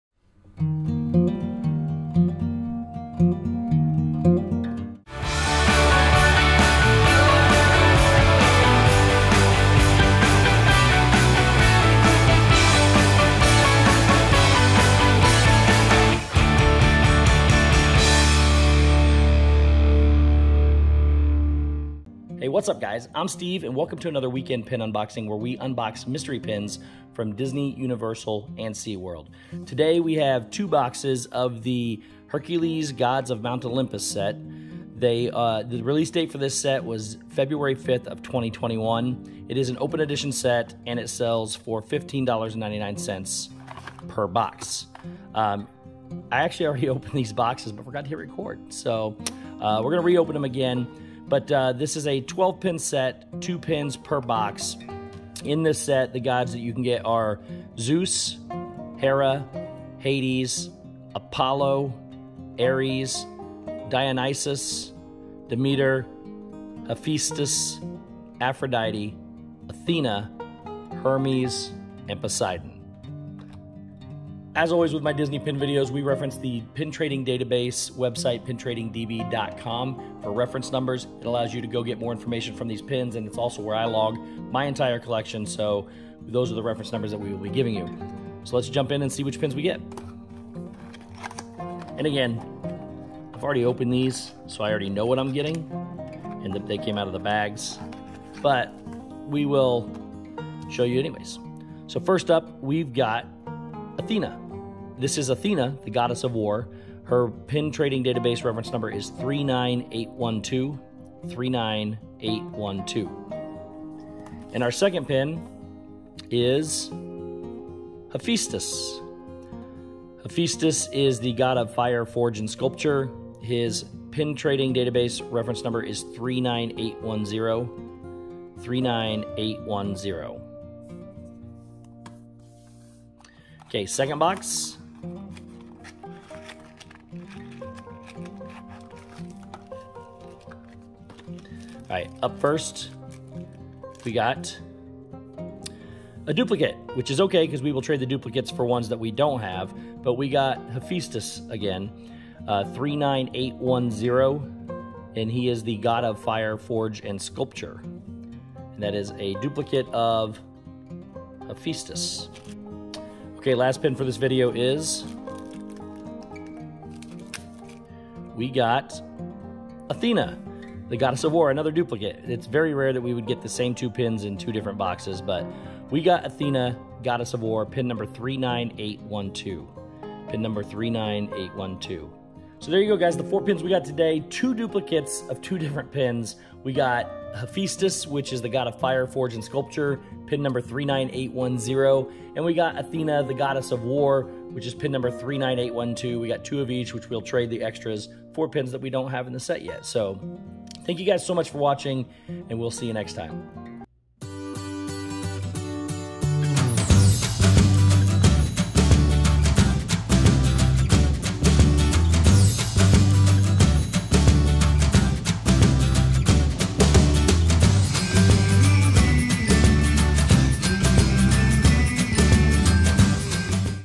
Disney Pin Unboxing | Hercules Gods of Mount Olympus
Today's Mystery Pin Box opening is from Disney and is the Hercules Gods of Mount Olympus Mystery pin set. Please join me every weekend for more Mystery Pin Unboxings from Disney World, Seaworld, and Universal!

stevesworld
12386 State Road 535 #404